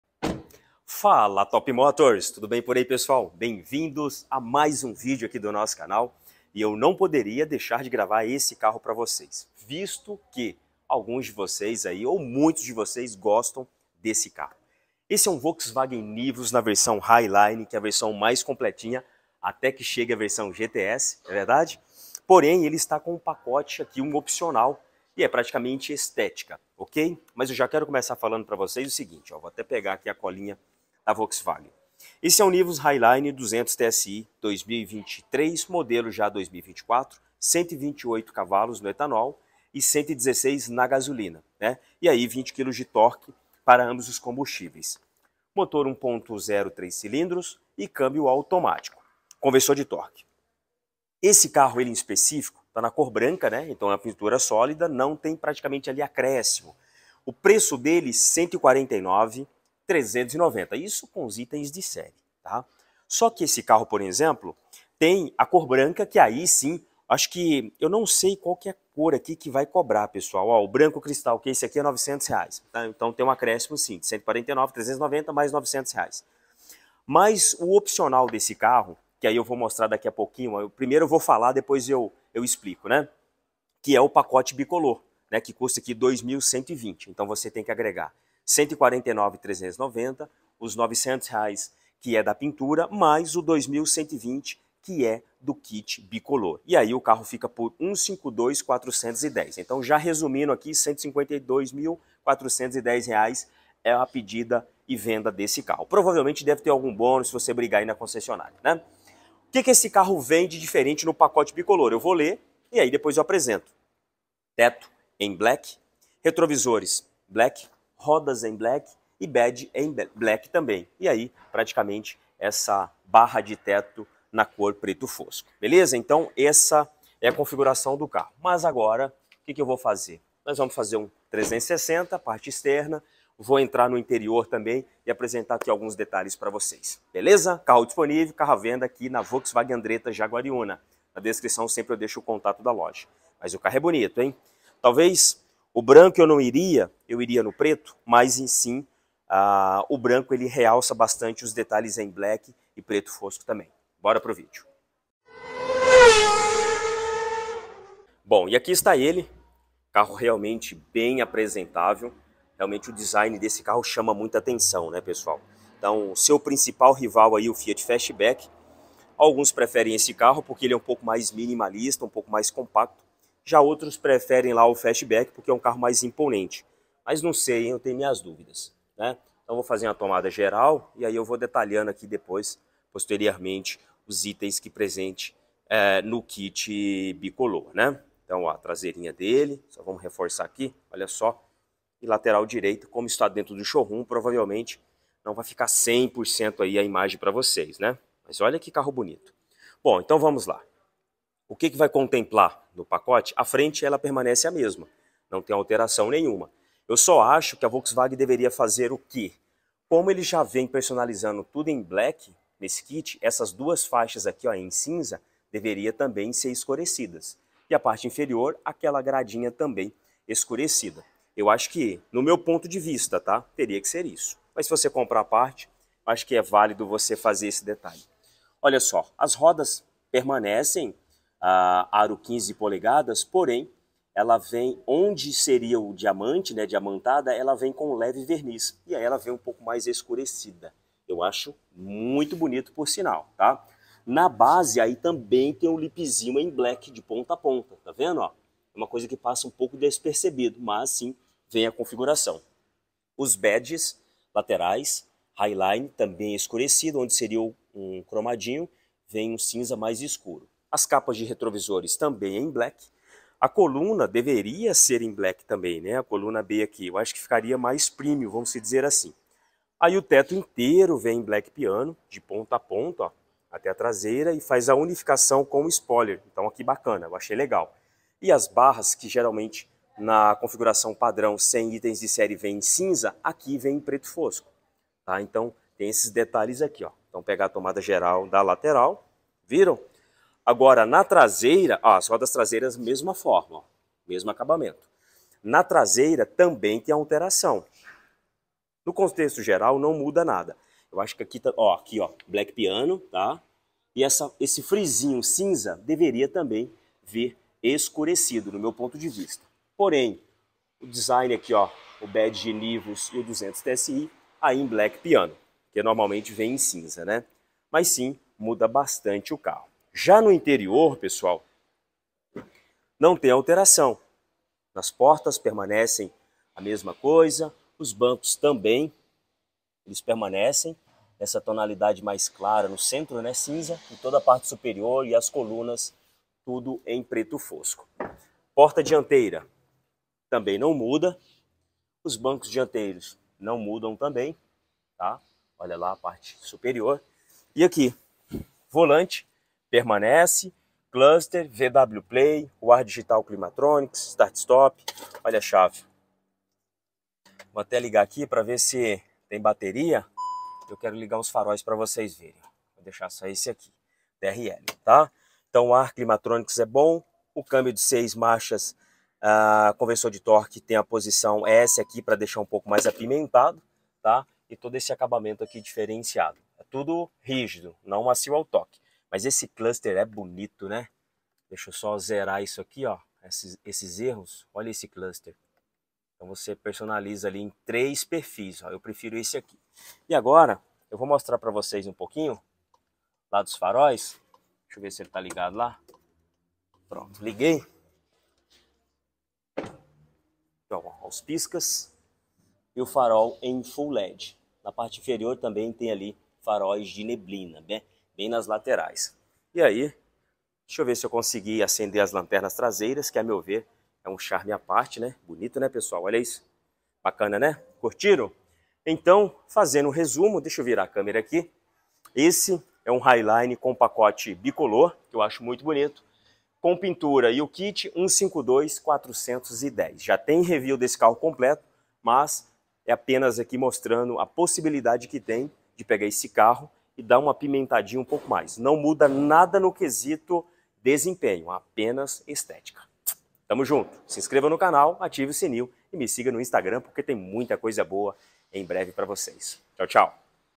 NIVUS HIGHLINE 2024 com o pacote BICOLOR
Essa unidade está com o kit especial, muda significativamente o carro.